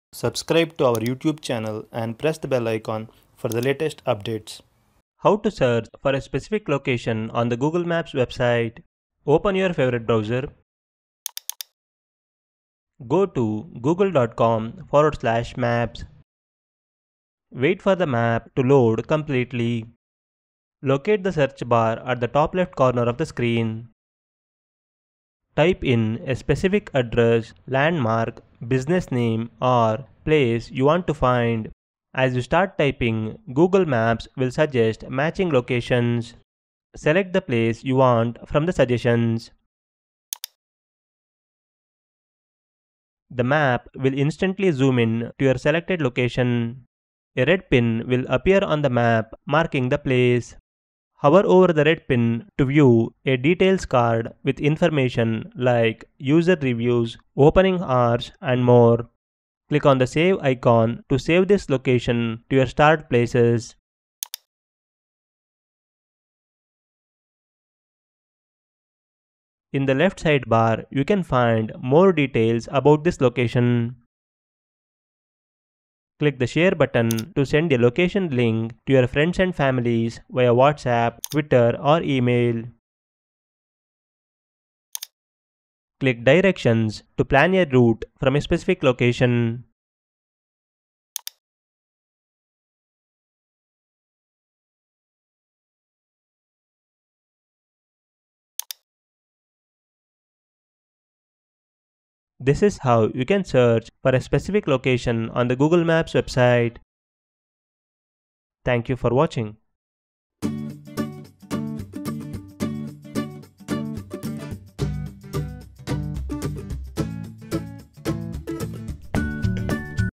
How to Search for a Specific Location on the Google Maps Website
0:00 Subscribe to Our Youtube Channel and Press the Bell Icon for Latest Updates
0:05 How to Search for a Specific Location on the Google Maps Website.
0:09 Open Your Favorite Web Browser.
0:12 Go to
0:17 Wait for the map to load completely.
0:20 Locate the Search Bar, at the top-left corner of the screen.
0:25 Type in a specific address, landmark, business name, or place you want to find.
0:31 As you start typing, Google Maps will suggest matching locations.
0:36 Select the place you want from the suggestions.
0:43 The map will instantly zoom in to your selected location.
0:47 A red pin will appear on the map marking the place.
0:51 Hover over the red pin to view a details card with information like user reviews, opening hours, and more.
0:59 Click on the “Save” Icon to Save this Location to Your Starred Places.
1:09 In the Left Side Bar, You Can Find More Details About this Location.
1:16 Click the “Share” button to send a location link to friends or family via WhatsApp, X (Twitter), or Email.
1:25 Click “Directions” to plan a route From a Specific Location.
1:42 This is How You Can Search for a Specific Location on the Google Maps Website.
1:48 Thank You for Watching.

Translations